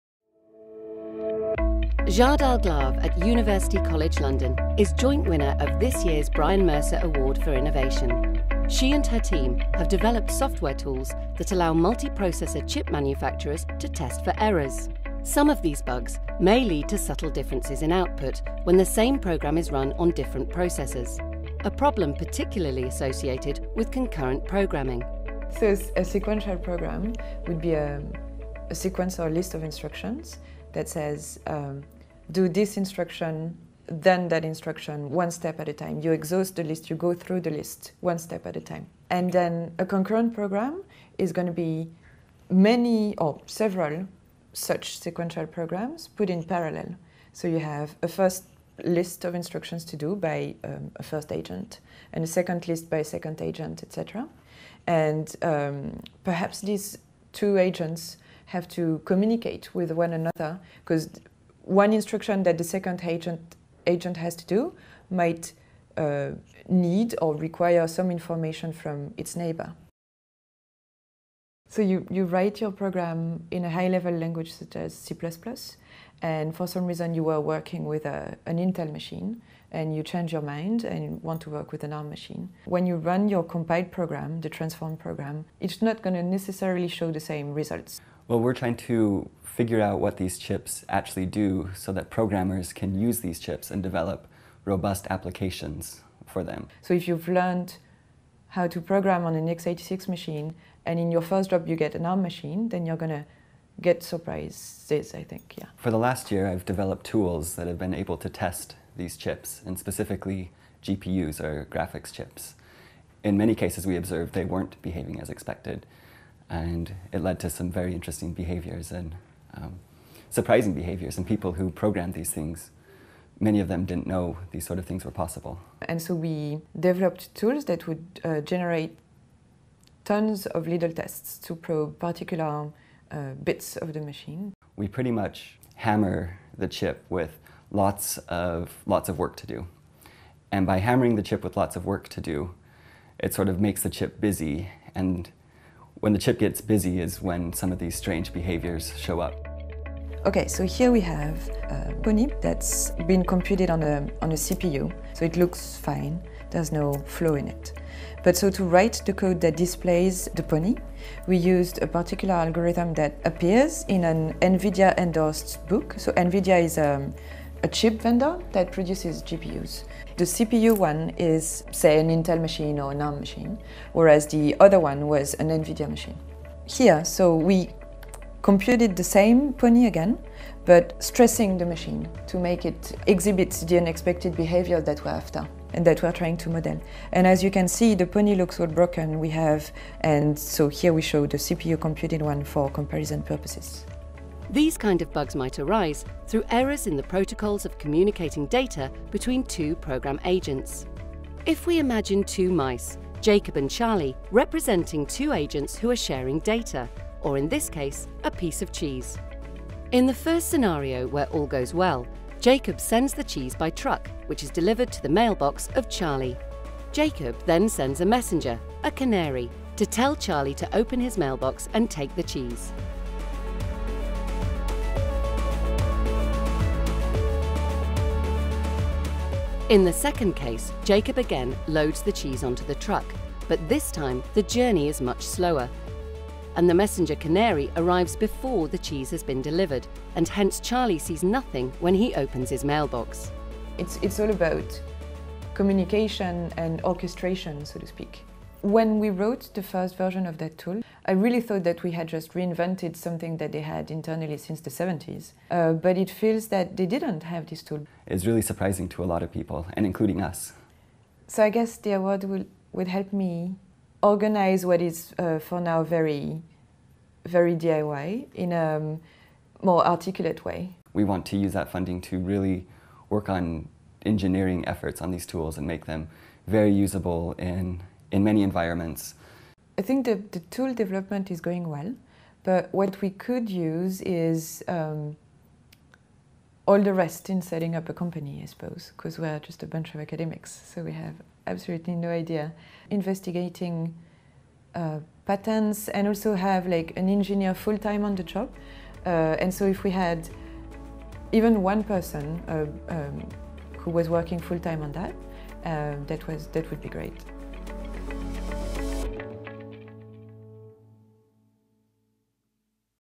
Royal Society Brian Mercer Award 2014 for innovation
Dr Jade Alglave, University College London, is the recipient of the 2014 Brian Mercer Award for Innovation for '.cat: a standard verification format for hardware chips'.

The Brian Mercer Award for Innovation helps to promote innovation and fill the funding gap between scientific research and the exploitation of an idea